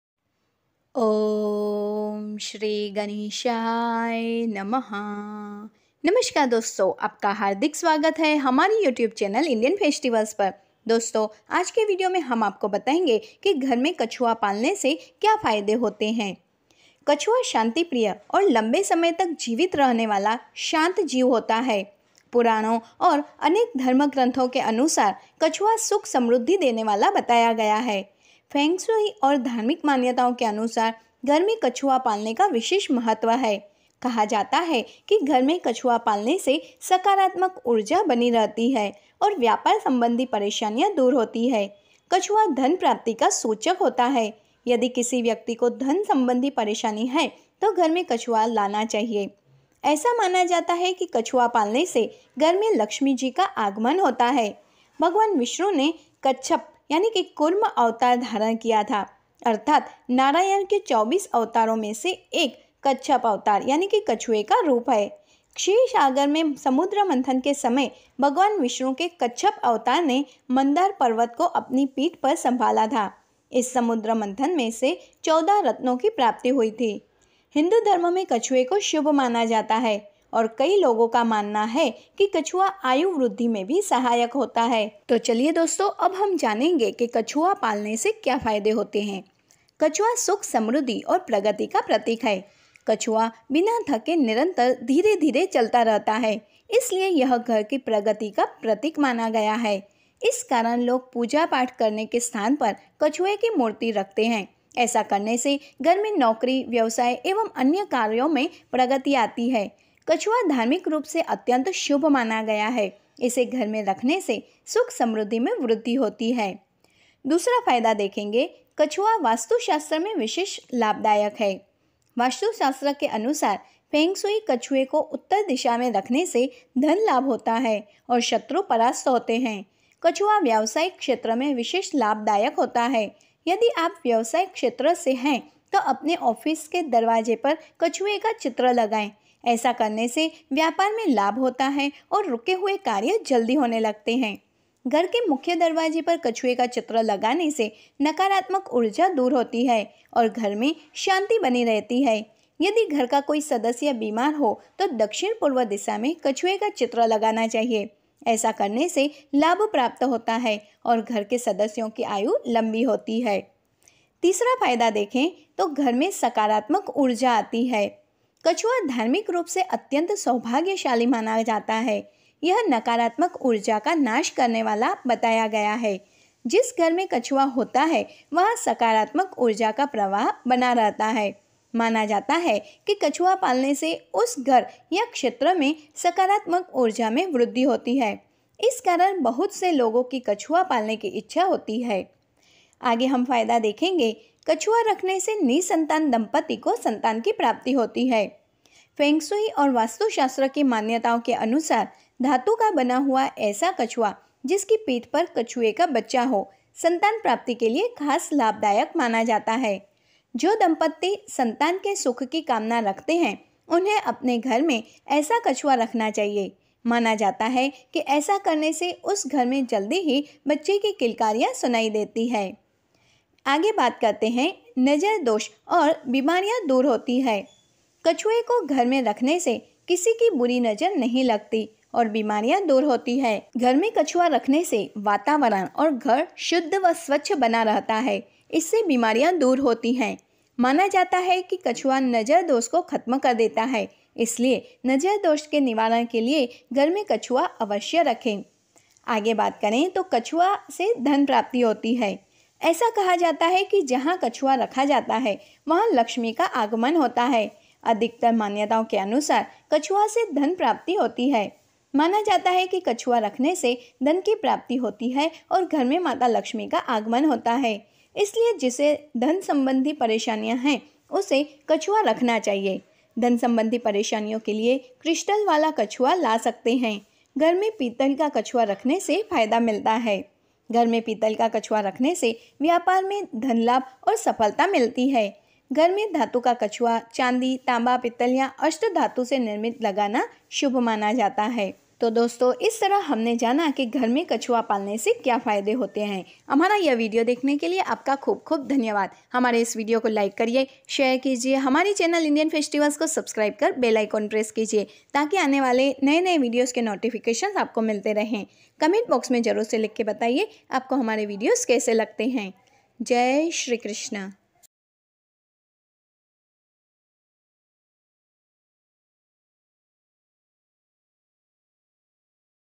कछुआ पालने के चमत्कारीक फायदे । देशी कछुआ क्या खाता है । Star Turtle Benefits
कछुए को घर में रखने के क्या फ़ायदे होते हैं।। देशी कछुआ क्या खाता है।। @FestivalofIndian    || Jay Chetwani || Bhumi Nanwani